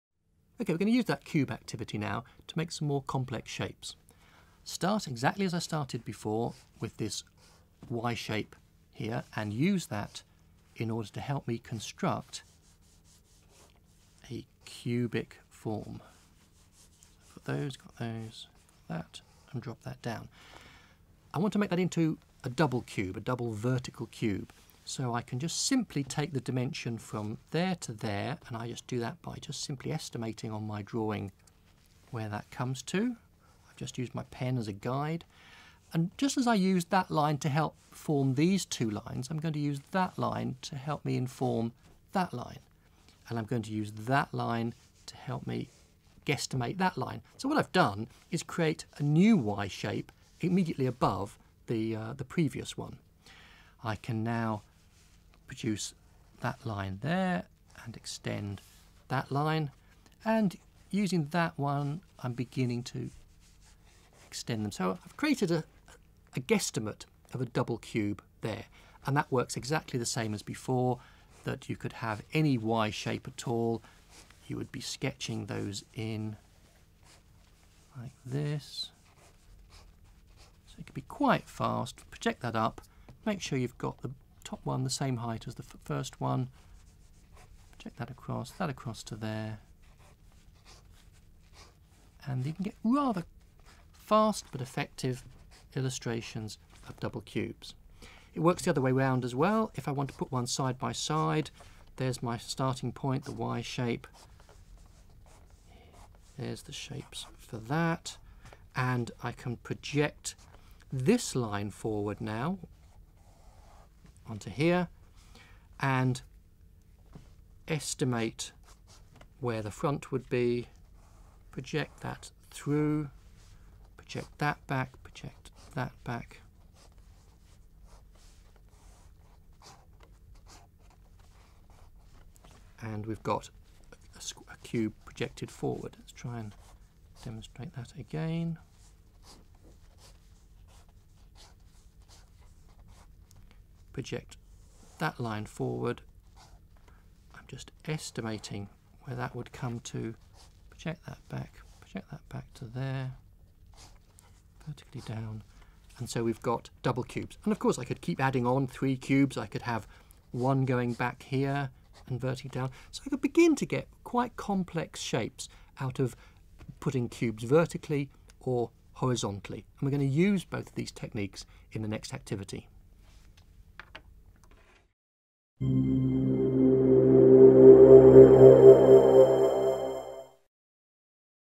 Cubic designs - Drawing, sketching and designing (6/19)
Using the ‘Y’ technique to extend cubic designs when creating a projection.